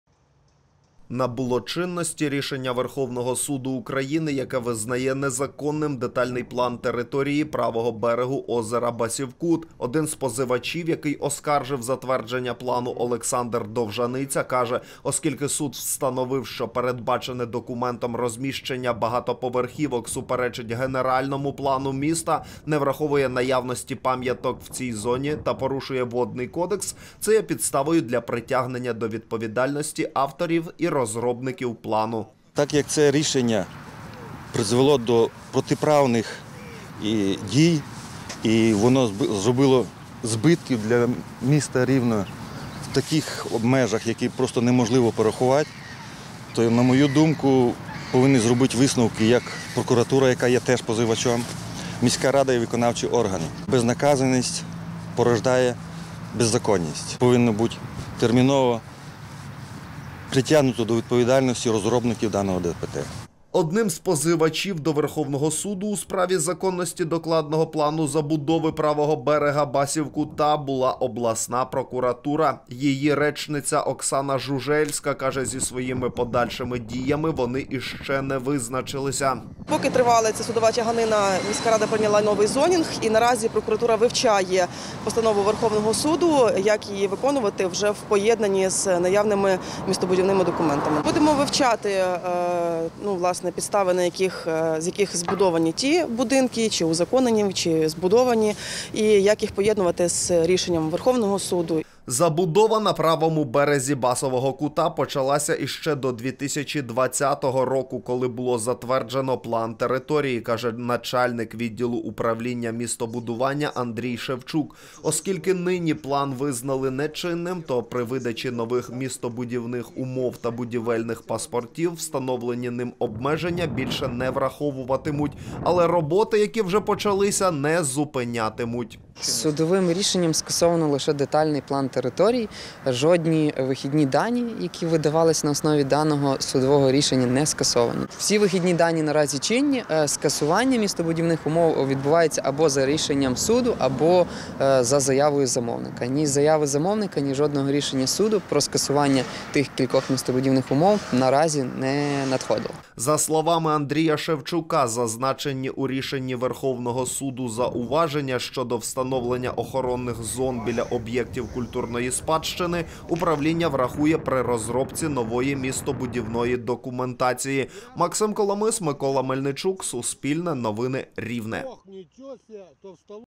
Незаконність ДПТ берега Басівкутського озера. Як це вплине на будівництво поблизу озера
Рішення Верховного Суду України про незаконність детального плану території (ДПТ) правого берега Басівкутського водосховища в Рівному є підставою притягнути до відповідальності його розробників. Про у повідомив один з позивачів Олександр Довжаниця. Рішення ВСУ — остаточне і оскарженню не підлягає. Суспільне з'ясовувало, як це вплине на будівництво поблизу озера.